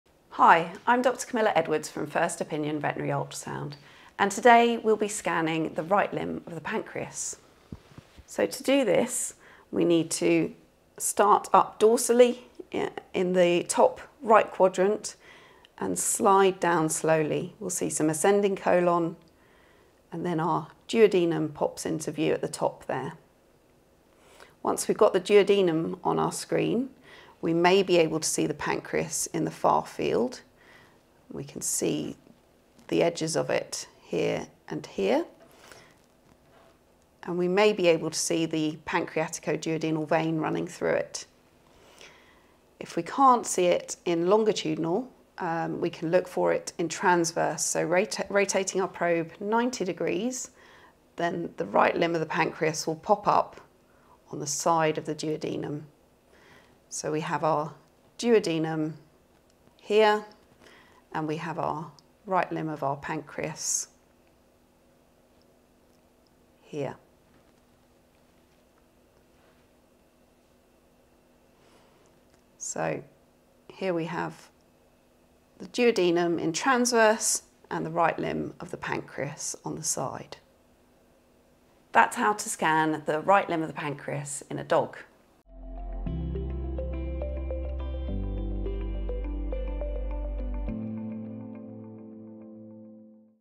Canine Pancreas - Right Limb
Dr. Edwards demonstrates patient positioning and key landmarks to identify the right limb of the pancreas in a dog.